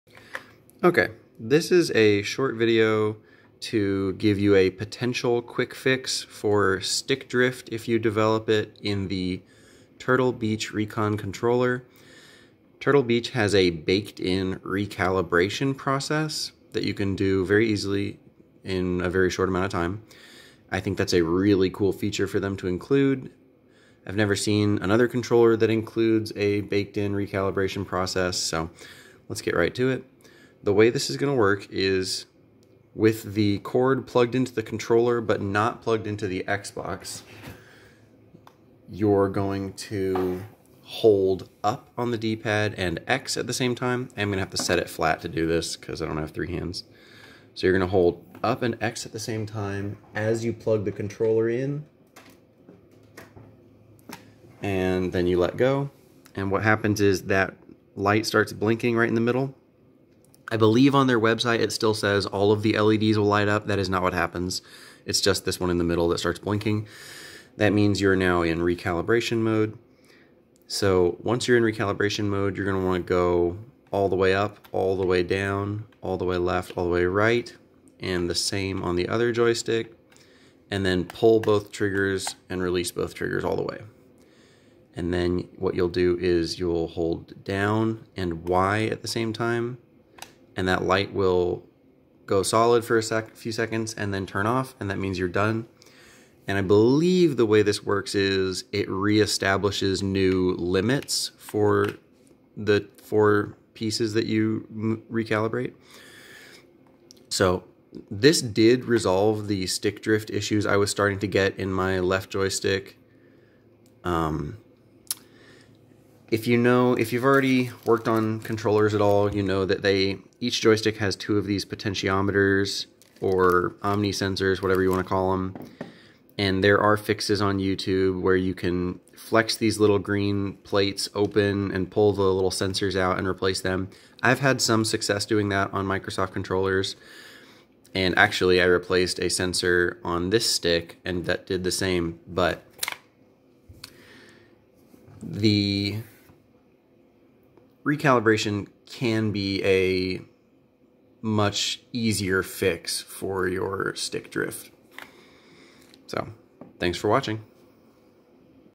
Turtle Beach Xbox One Recon Controller Stick Drift Potential Fix (Recalibration/Reboot Tutorial)
If you game a lot, you've probably encountered stick drift. While a frustrating and sometimes unfixable issue, if you're gaming with a Turtle Beach Recon Controller, there is one very easy potential fix you can try. The Recon controller has a built-in or baked-in recalibration process that I've had some success using to eliminate stick drift and some other glitches.
Before you void your warranty by cracking this thing open, give this a try!